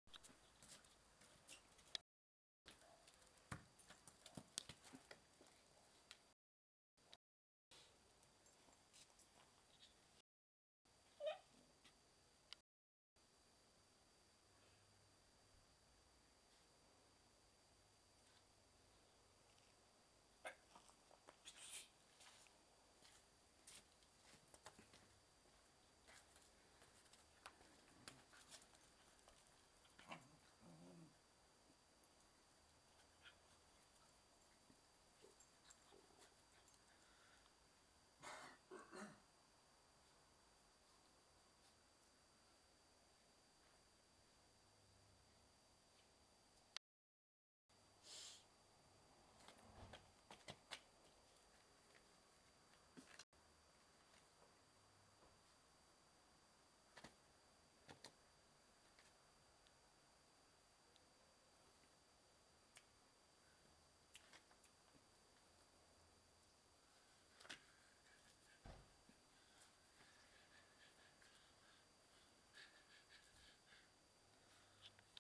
More of my Cairn terriers playing
Bailey 2 years and Ruby 10 weeks playing